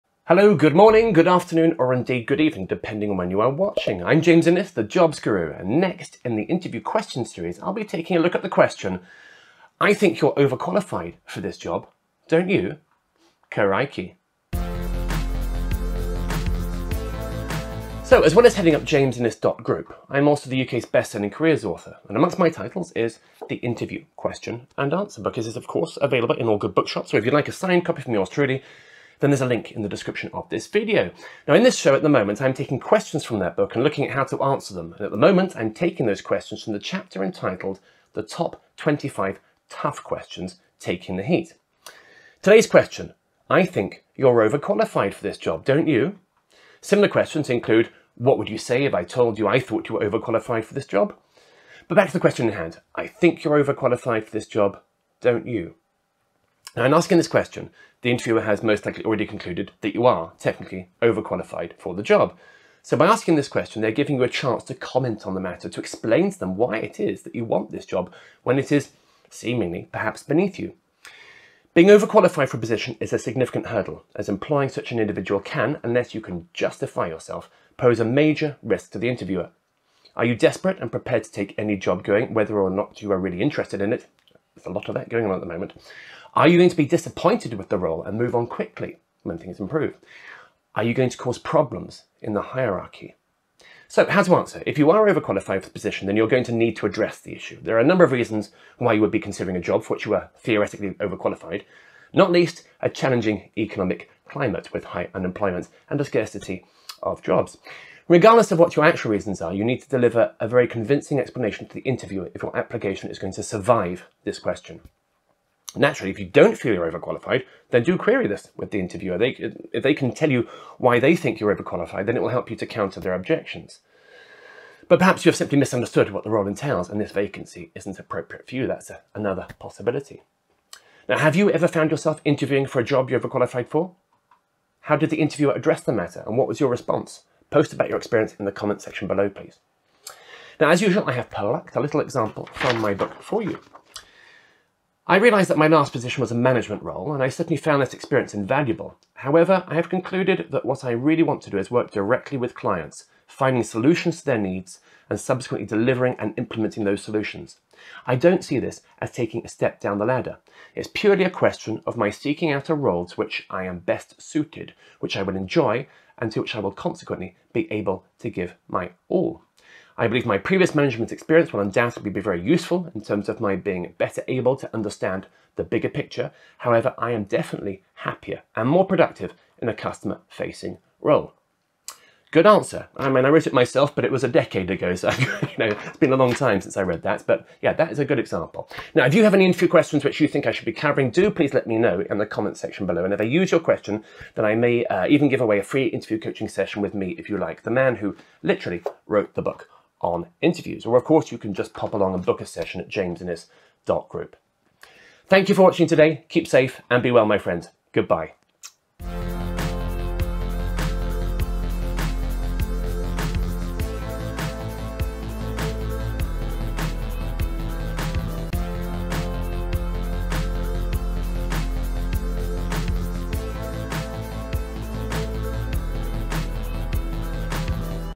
What Happens When The Interviewer Asks If You Are Overqualified?
If you are overqualified for the position, then you’re going to need to address the issue. There are various reasons why you would be considering a job for which you are theoretically overqualified – not least a challenging economic climate with high unemployment and a scarcity of jobs. Regardless of what your actual reasons are, you need to deliver a very convincing explanation to the interviewer if your application is to survive this question.

✩ Today’s Question ✩
‘I think you’re overqualified for this job. Don’t you?’
Similar questions include:
What would you say if I told you I thought you were overqualified for this job?
Won’t you get bored doing this?

00:00 Intro
00:42 Are you overqualified for the position?
01:33 How to answer
02:20 Example
03:03 Outro

✭ The CV Book
✭ The Cover Letter Book
✭ The Interview Book
✭ The Interview Question & Answer Book
✭ Ultimate New Job